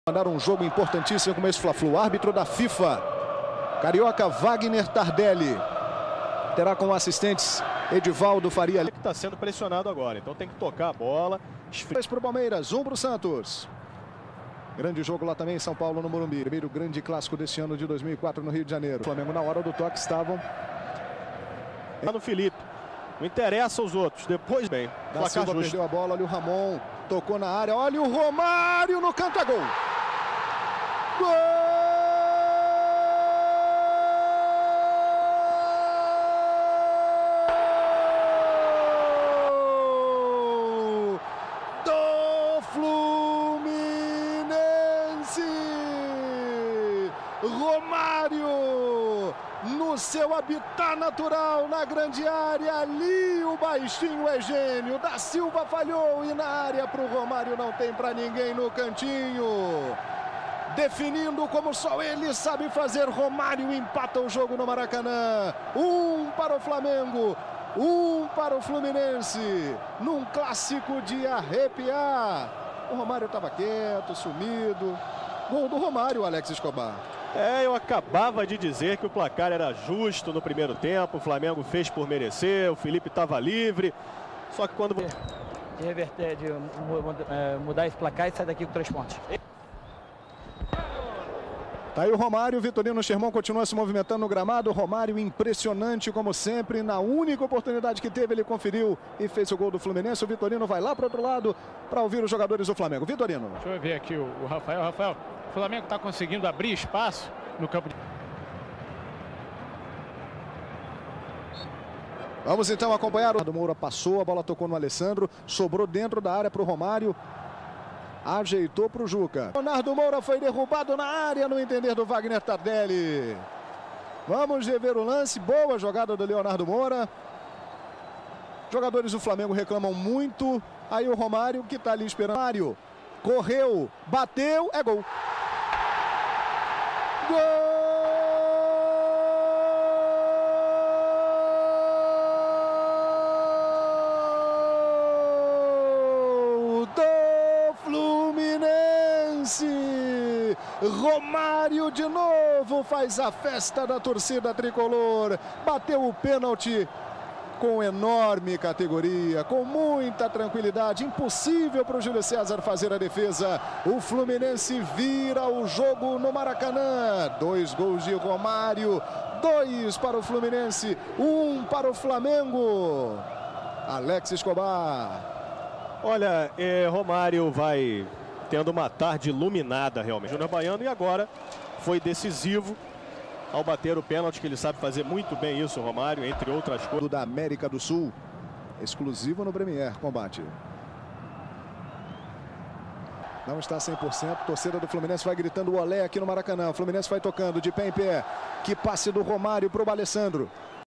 Romário x Flamengo (Campeonato Carioca) 2004
Romário faz 2 gols.